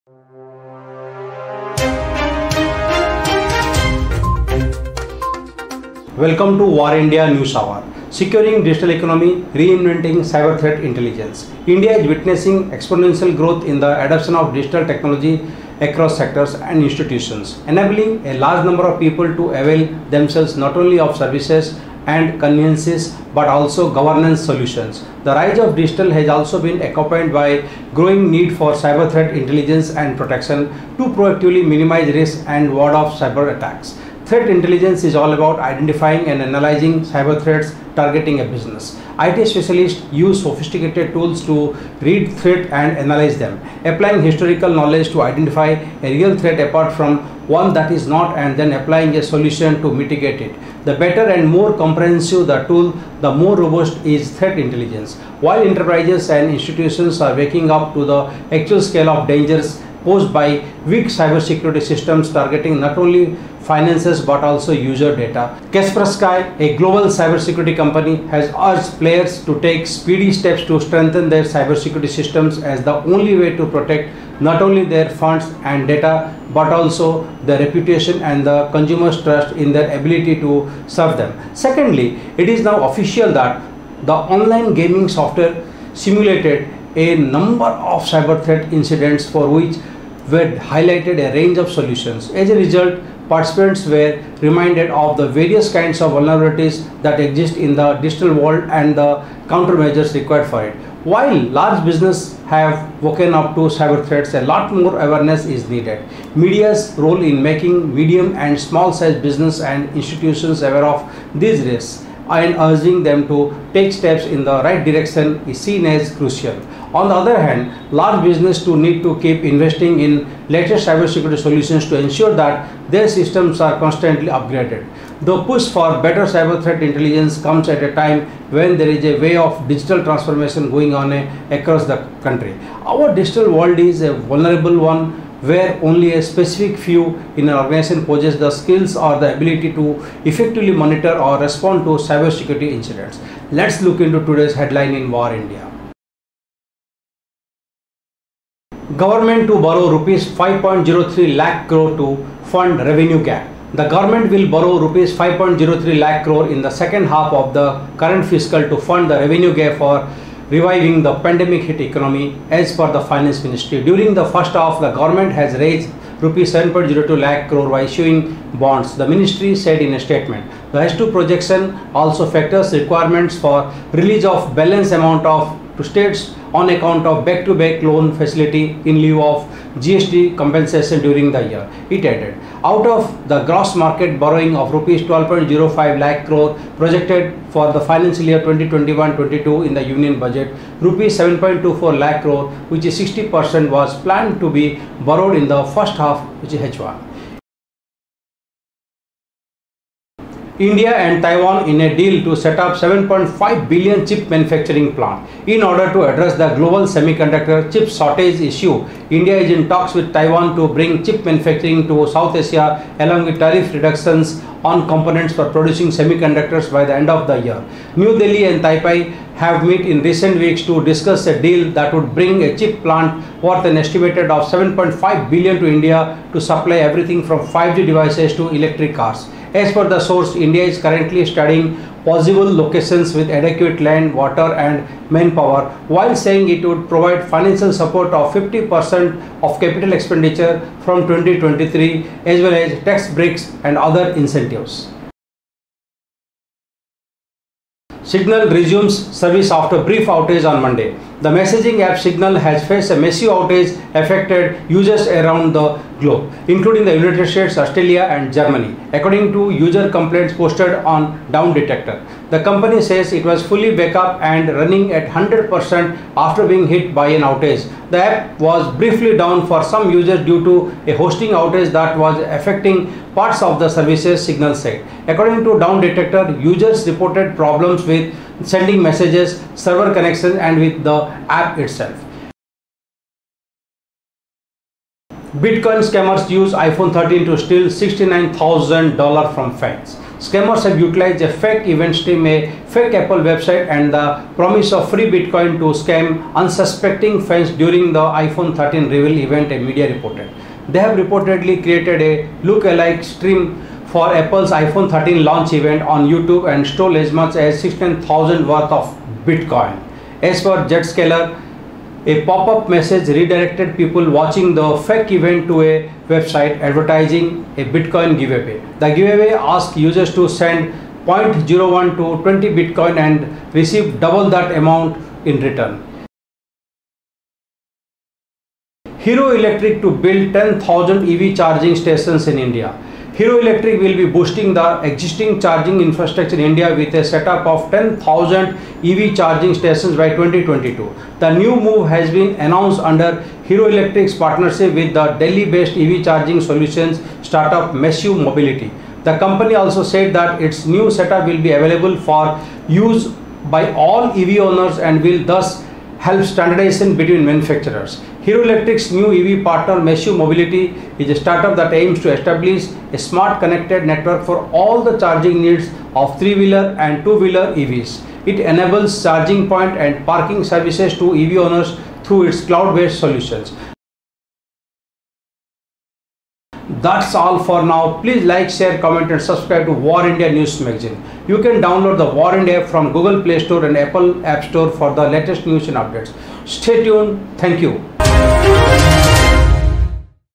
Securing digital economy: Reinventing cyberthreat intelligence
The digital world is vulnerable

India is witnessing exponential growth in the adoption of digital technology across sectors and institutions, enabling a large number of people to avail themselves not only of services and conveniences but also governance solutions. The rise of digital has also been accompanied by growing need for cyberthreat intelligence and protection, to proactively minimize risks and ward off cyberattacks. Threat intelligence is about identifying and analyzing cyberthreats targeting a business. IT specialists use sophisticated tools to read threats and analyse them, applying historical knowledge to identify a real threat apart from one that is not, and then applying a solution to mitigate it. The better and more comprehensive the tool, the more robust is threat intelligence.  While enterprises and institutions are waking up to the actual scale of dangers posed by weak cybersecurity systems targeting not only finances but also user data, Kaspersky, a global cybersecurity company, has urged players to take speedy steps to strengthen their cybersecurity systems as the only way to protect not only their funds and data but also their reputation and the consumer’s trust in their ability to service them.

Secondly, it is now official that, the online gaming software simulated a number of cyberthreat incidents, for which were highlighted a range of solutions. As a result, participants were reminded of the various kinds of vulnerabilities that exist in the digital world and the counter-measures required for it. While large businesses have woken up to cyberthreats, a lot more awareness is needed. Media’s role in making medium- and small-sized businesses and institutions aware of these risks and urging them to take steps in the right direction is seen as crucial. On the other hand, large businesses too need to keep investing in latest cybersecurity solutions to ensure that their systems are constantly upgraded. The push for better cyberthreat intelligence comes at a time when there is a wave of digital transformation going on across the country. Our digital world is a vulnerable one where only a specific few in an organization possess the skills or the ability to effectively monitor or respond to cybersecurity incidents.

0:00 Intro
2:28 Govt to borrow Rs 5.03 lakh cr to fund revenue gap
3:19 India and Taiwan in a deal to Set Up $7.5 bn
4:07 Signal resumes service after brief outage on Monday '
4:44 Bitcoin scammers used iphone13 to steal $69,000 from fans
5:28 Heroelectric to build 10,000 EVchargingstations in India